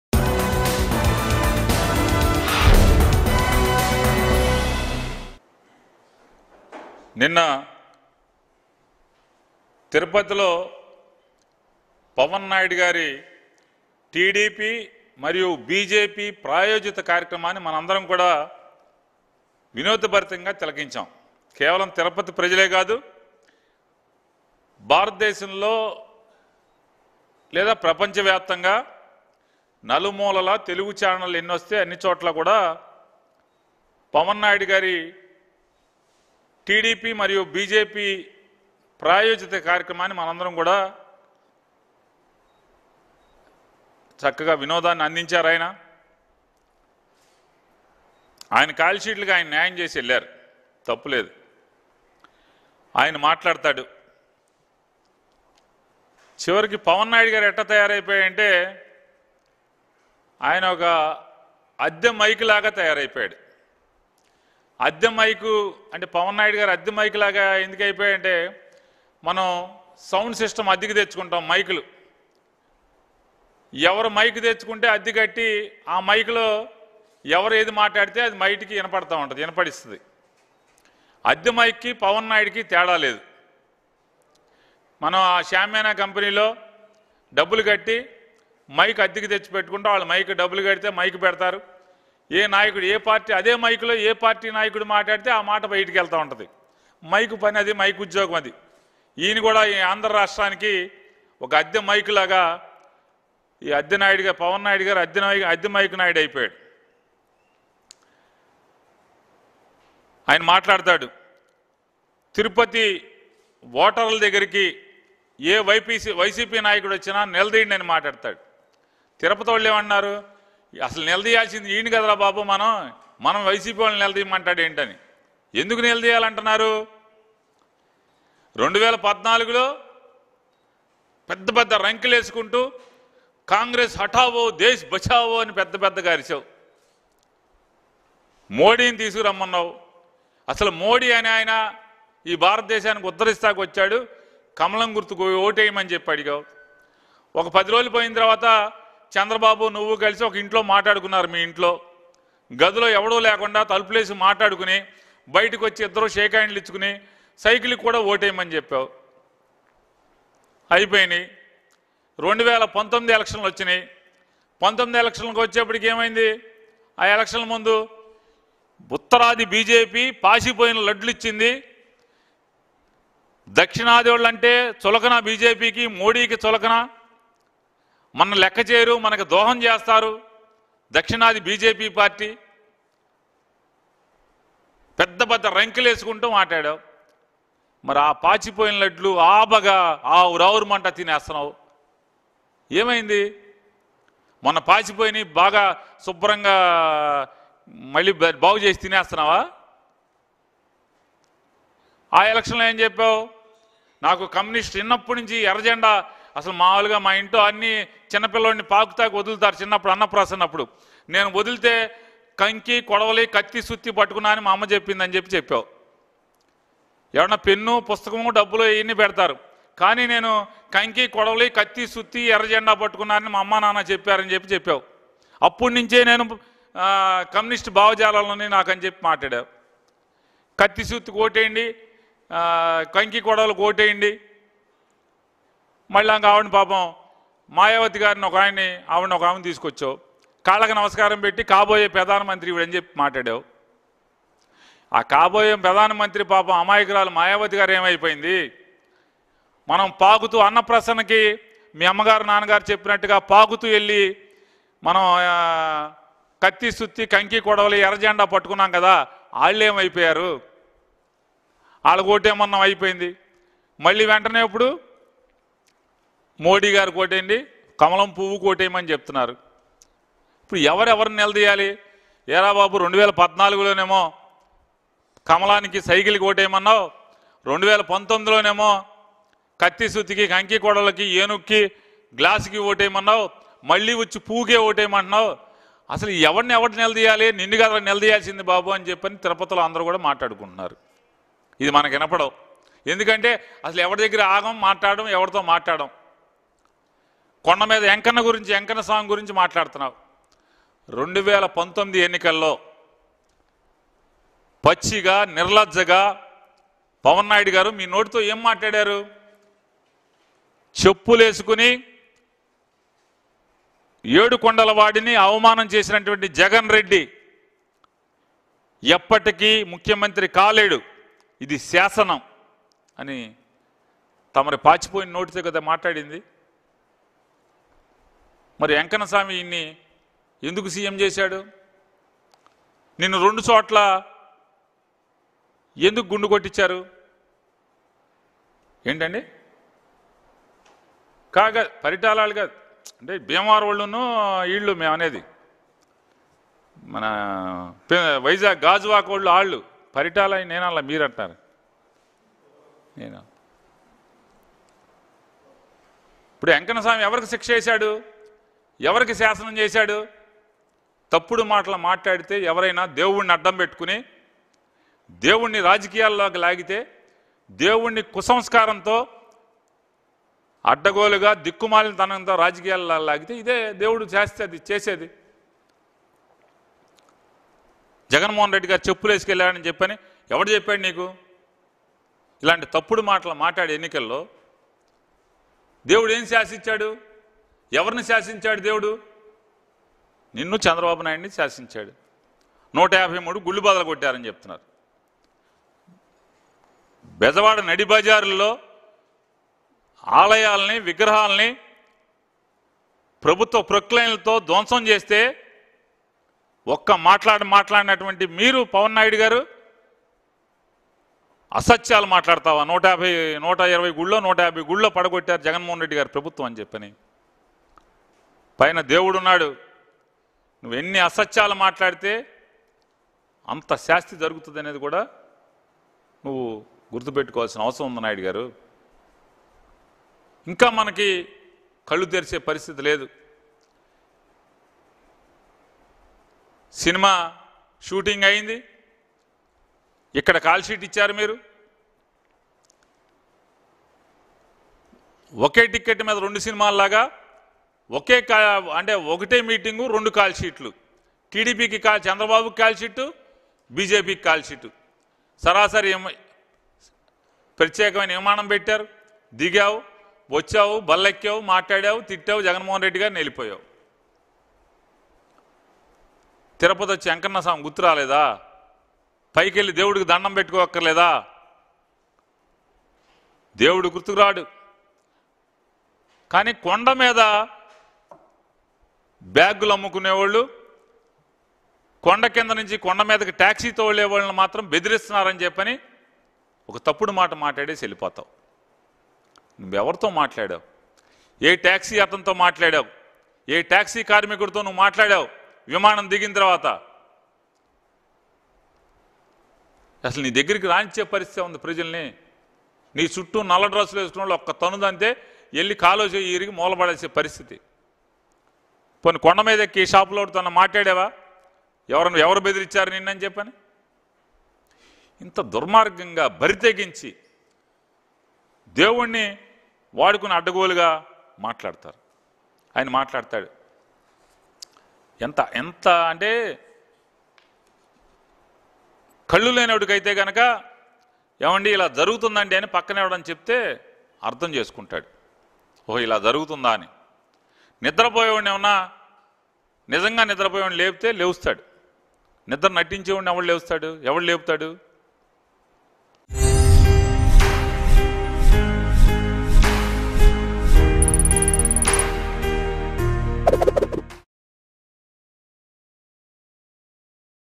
Minister Perni Nani Satirical Comments On Janasena Chief Pawan Kalyan | YCP Vs Janasena | Mango News
Minister Perni Nani Satirical Comments On Janasena Chief Pawan Kalyan.